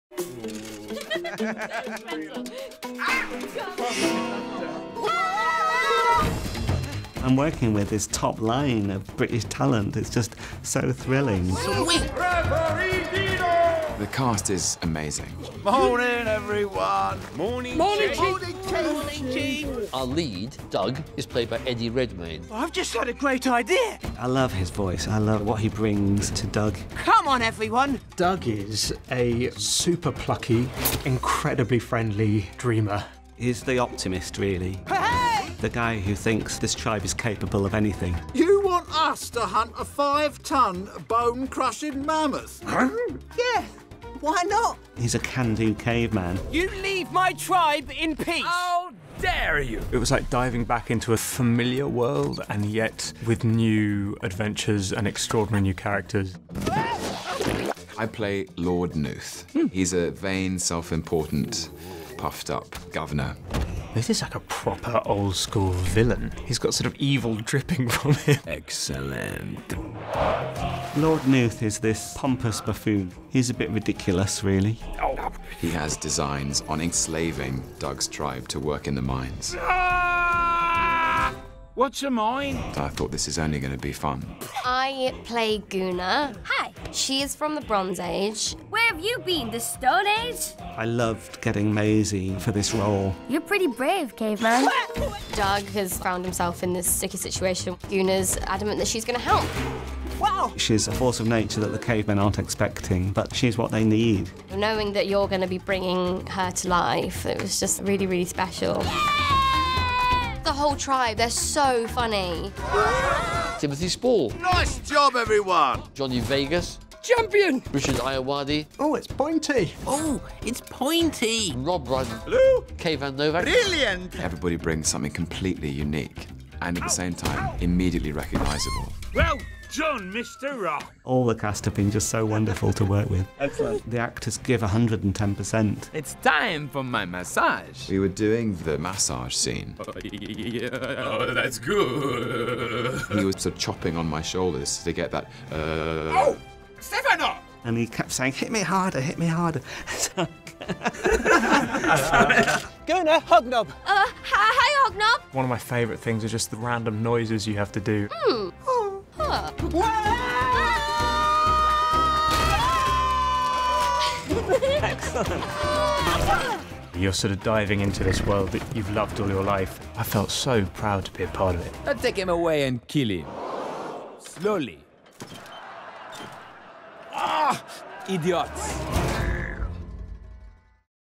Early Man - Eddie Redmayne, Tom Hiddleston and Maisie Williams
The stars of Early Man - Eddie Redmayne (Dug), Tom Hiddleston (Lord Nooth) and Maisie Williams (Goona) - talk about voicing their characters. We also hear from producers Nick Park and Peter Lord.